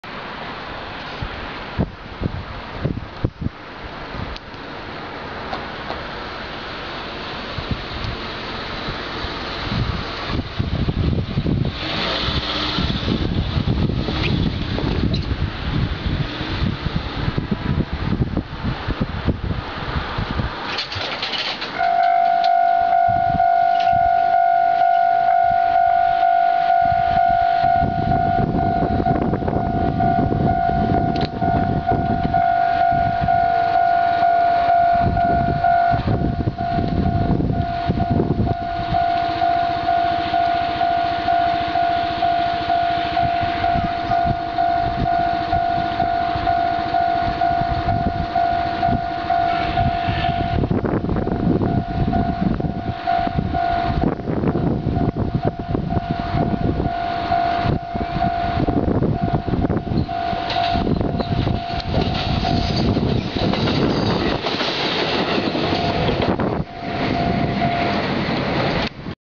2011/4/2 普通列車(キハ220)走行@日田～光岡間
2011/4/2　JR久大本線日田～光岡間を走行する久留米行き普通列車(キハ220_1両)を撮影しました。